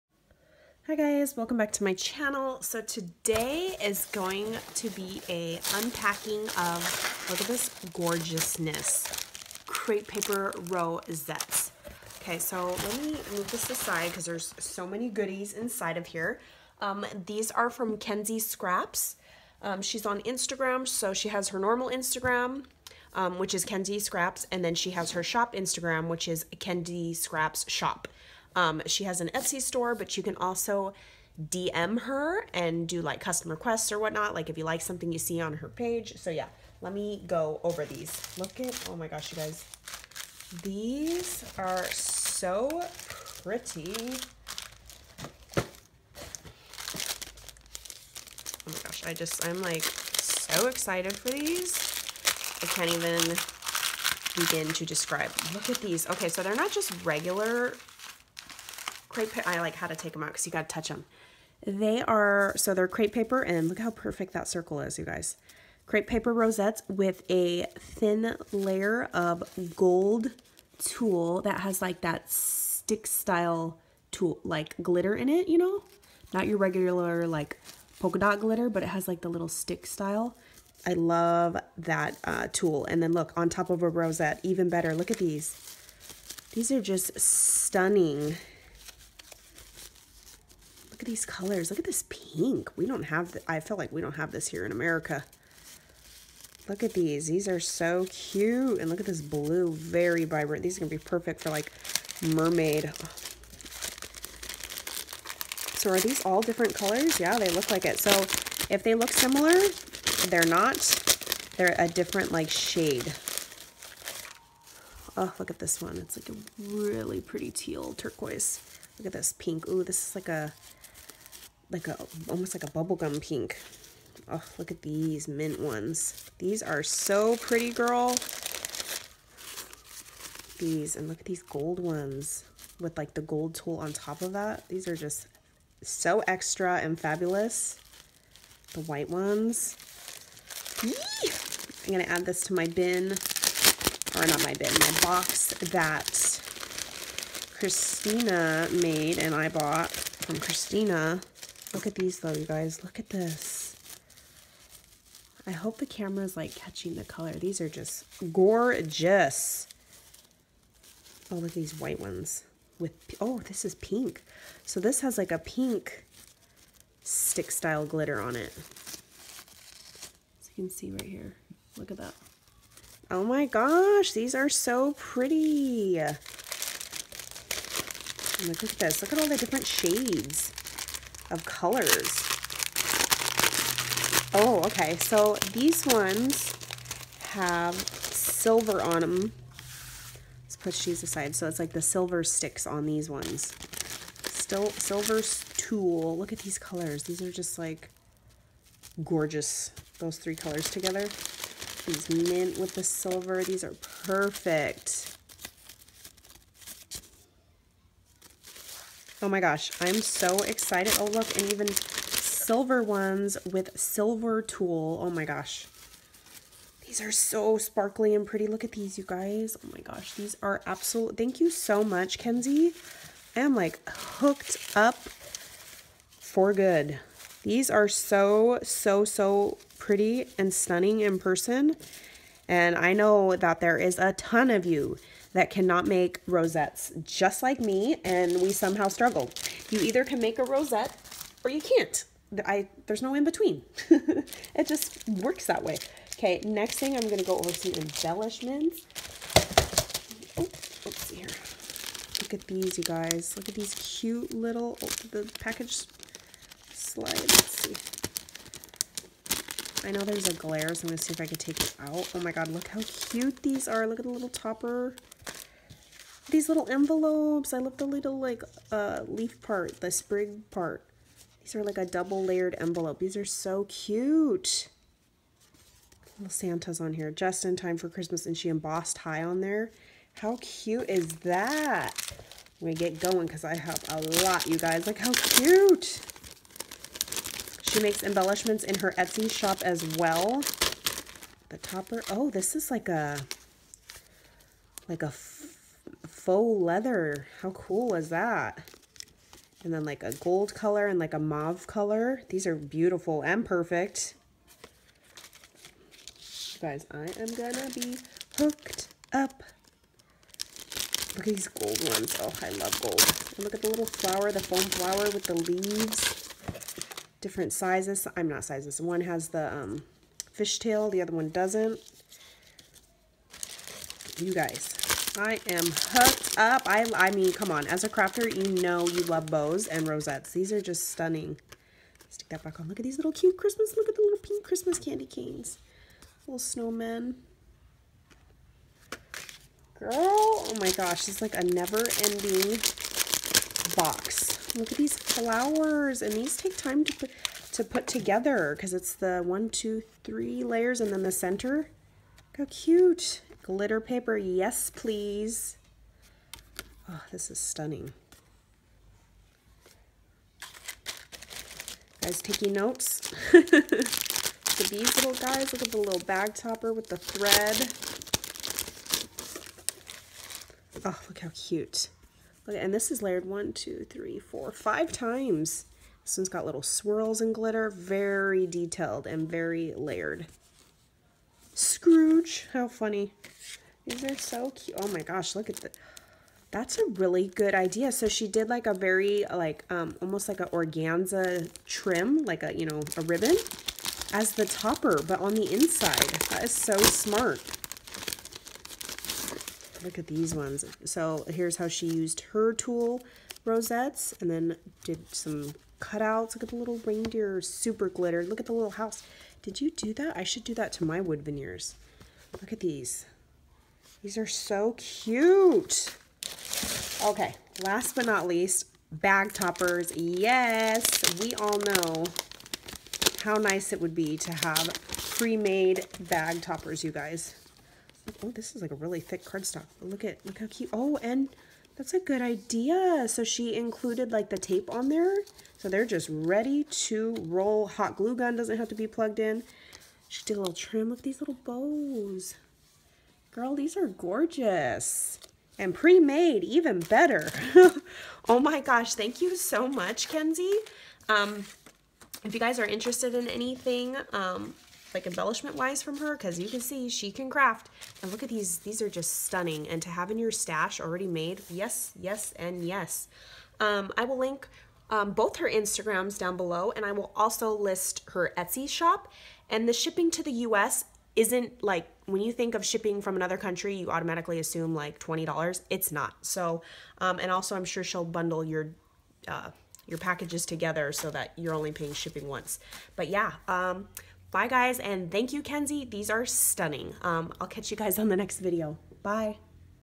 Incoming Embellishments and rosettes from KENZISCRAPS
Thank you sooo so.much Kenzi, I love it all! So much time And attention to detail, thank you for being so generous and sweet to me💕💕💕 i truly appreciate it!

Xoxox
Toni